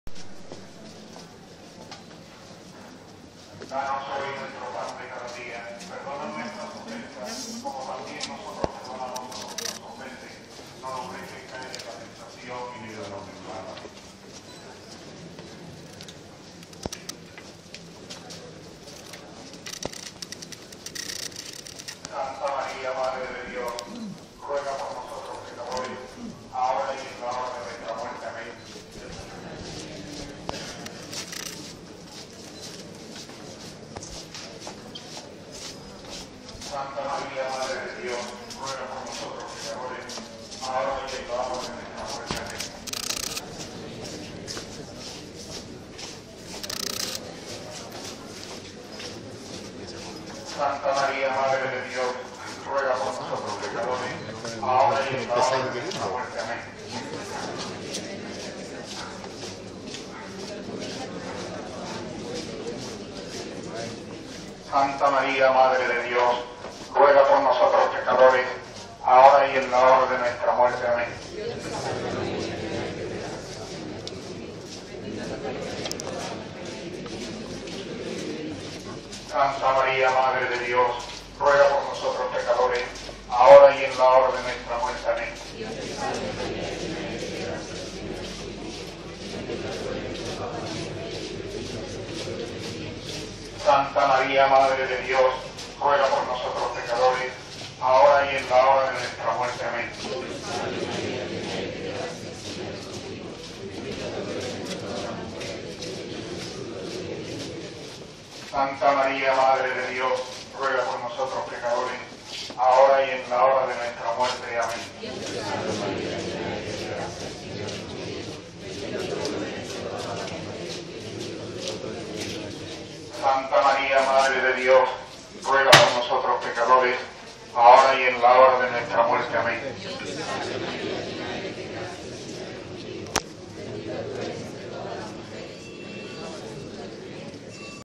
ROSARIO DE LA STMA VIRGEN DE LAS TRISTEZAS DE LA VERA+CRUZ, 21-9-14. (III)
Rosario de la Stma Virgen de las Tristezas de la Hdad de la Vera+Cruz en su traslado al Convento de Sta Rosalía.